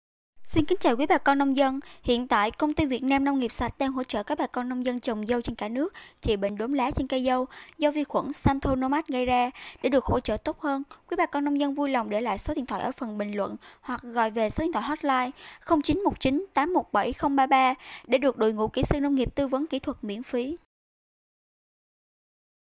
KỸ THUẬT XỬ LÝ BỆNH ĐỐM LÁ TRÊN CÂY DÂU TÂY
VIỆT NAM NÔNG NGHIỆP SẠCH

Thuốc bvtv _ hạt giống_ dụng cụ nông nghiệp

📲📲Hỗ Tư vấn kỹ thuật tại vườn📲📲

✅Tư vấn kỹ thuật : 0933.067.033